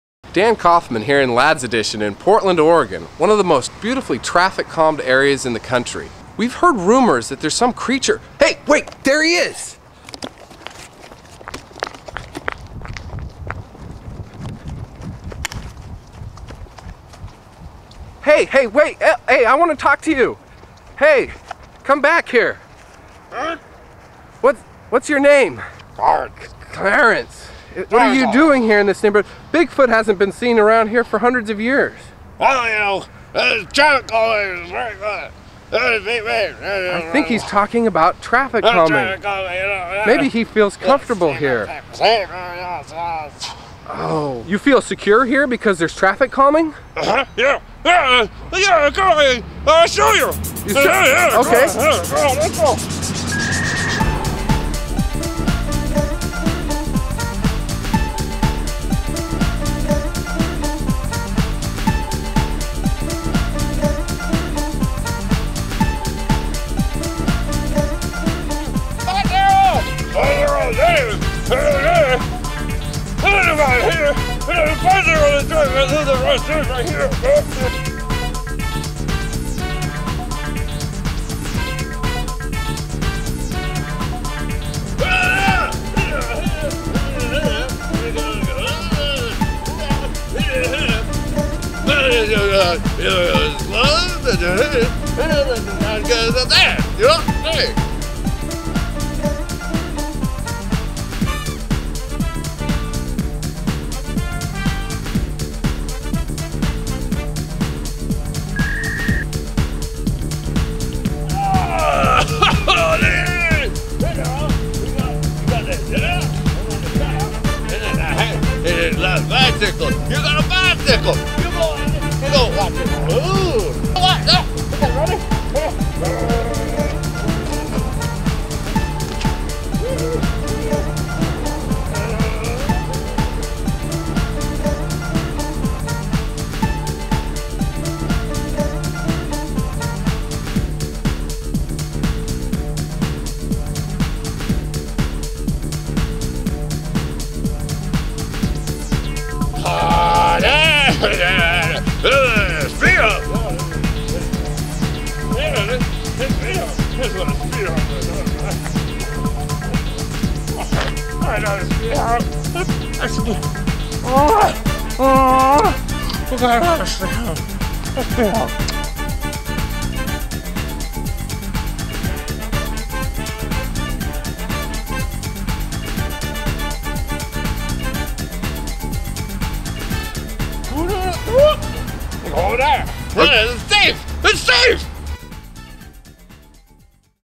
Clarence the Traffic Calming Sasquatch (2007: Portland, OR)
We could all use a good laugh. Did you ever know that Streetfilms' producer Clarence Eckerson, Jr. once donned a Sasquatch outfit to praise Portland, OR's safe streets programs, being one of the few bright lights folks in the USA had to look to for good biking and open space policies?

Well here is that video that hasn't been on any of the Streetfilms Youtube accounts ever!

How did this come about? My friend Dan Kaufman from Crank My Chain/PDXK-TV called me once while I was visiting and started out saying, "So I happen to have this bigfoot costume at my house for a few days. " and immediately ran over and the two of us came up with this hysterical silly bit that we practically invented on the go. It went very viral at that time with over 10k plays.

PLOT: Dan essentially goes searching for the long lost bigfoot in Ladd's Edition, an area well known for its traffic calming. He stumbles upon the ever-elusive, mythic character and finds that safer streets have coaxed him out of the wild.